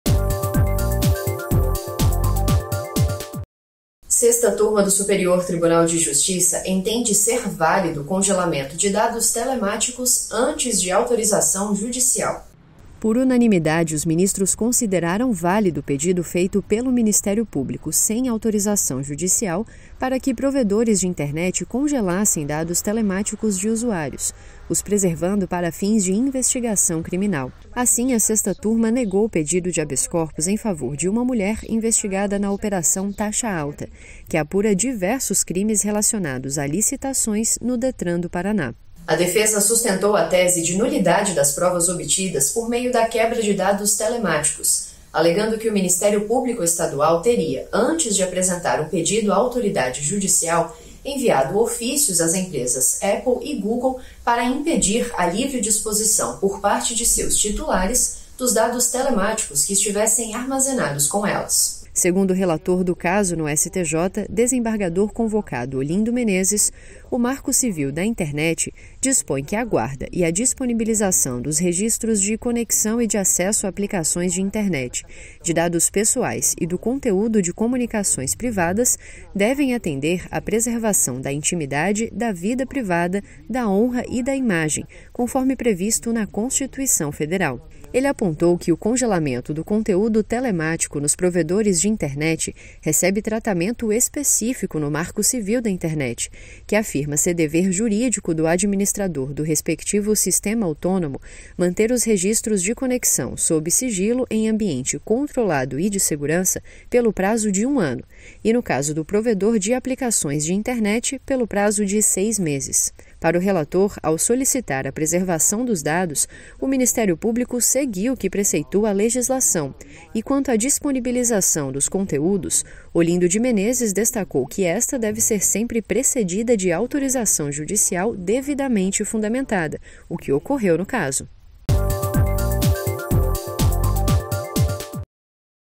É válido pedido de congelamento de dados telemáticos antes de autorização judicial 14.03.22
​Por unanimidade, a Sexta Turma do Superior Tribunal de Justiça (STJ) considerou válido o pedido feito pelo Ministério Público – sem autorização judicial – para que provedores de internet congelassem dados telemáticos de usuários, preservando-os para fins de investigação criminal.
O colegiado negou pedido de habeas corpus em favor de uma mulher investigada na Operação Taxa Alta, que apura diversos crimes relacionados a licitações no Detran do Paraná.